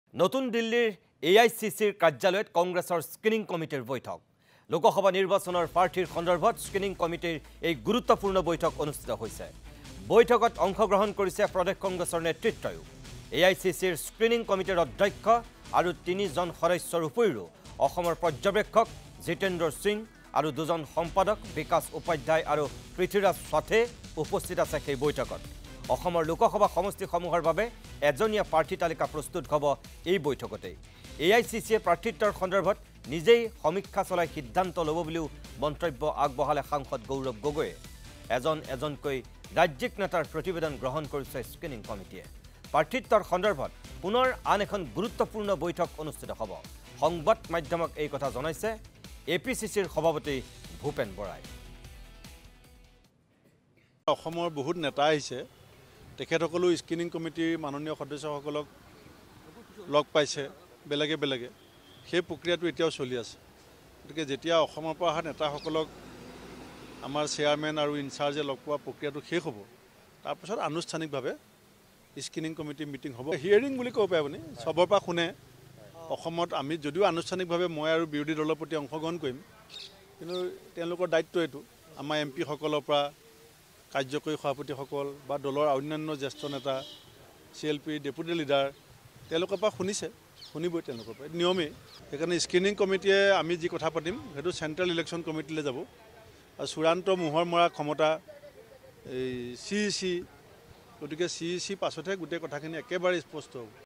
লোকসভা নিৰ্বাচনৰ প্ৰাৰ্থী সন্দৰ্ভত অনুষ্ঠিত হৈছে স্ক্ৰিনিং কমিটীৰ বৈঠক...
নতুন দিল্লীত AICCৰ স্ক্ৰিনিং কমিটীৰ বৈঠক। লোকসভা নিৰ্বাচনৰ প্ৰাৰ্থী সন্দৰ্ভত অনুষ্ঠিত হৈছে স্ক্ৰিনিং কমিটীৰ বৈঠক। বৈঠকত অংশগ্ৰহণ কৰিছে প্ৰদেশ কংগ্ৰেছৰ নেতৃত্বই। প্ৰাৰ্থিত্ব সন্দৰ্ভত পুনৰ আন এখন গুৰুত্বপূৰ্ণ বৈঠক হ'ব। সংবাদ মাধ্যমক জনালে APCCৰ সভাপতি ভূপেন বৰাই।